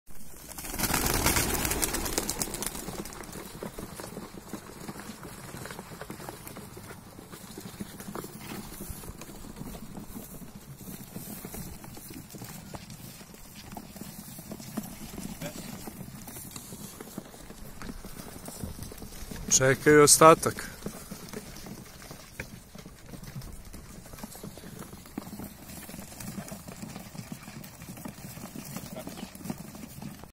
TRENING GOLUBA PISMONOŠA
Jedan kratki snimak puštanja golubova pismonoša tj pripremanje za dug put. Trening golubova pismonoša takozvanih poštara, u cik zore rane, kolega Neša ih je sve lepo spakovao ubacili ih u Golfa dvicu ljutu mašinicu pa pravac Pankovo ispraćamo ih za Požarevac. Šta mislite ko će pre stići mi sa Golf 2 ili golubovi? Podelite, lakujte, zapratite kanal @F45T88TV i naravno uživajte u životu.